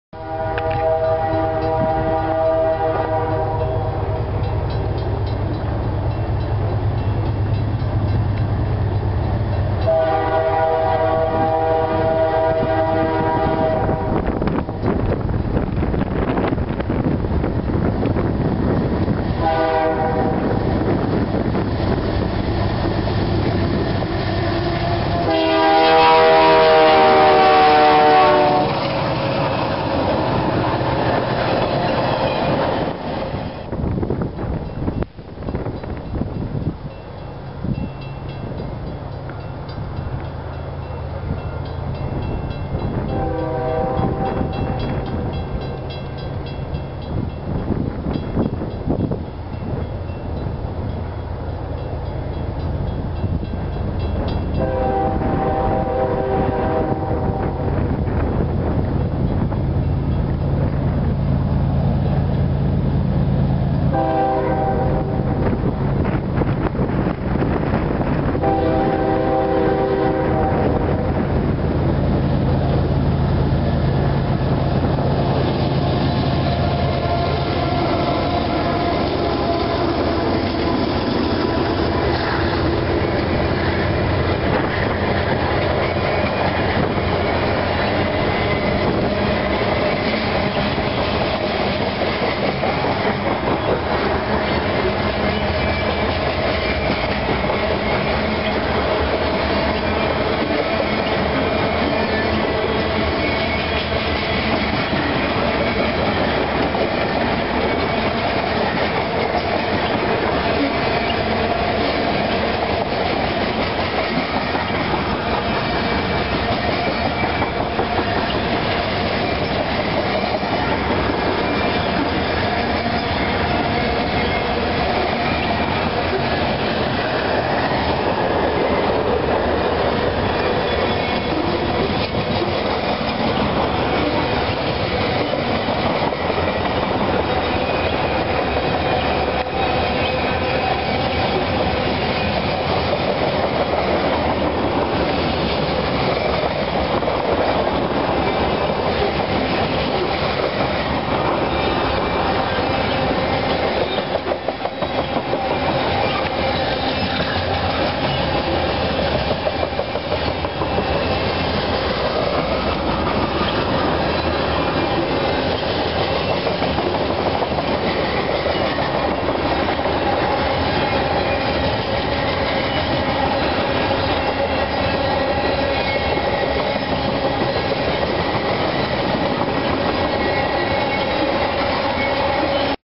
CP Rail in Banff, Alberta, April 2008
A couple of quick clips of two Westbound Freight Trains heading into the town of Banff on the east side. First one has a CEFX GE AC4400CW leading the consist. Horn has a nice echo to it in the mountains and also made my friend who was recording jump quite nicely. Made me jump as well while trying to take photos of this train too!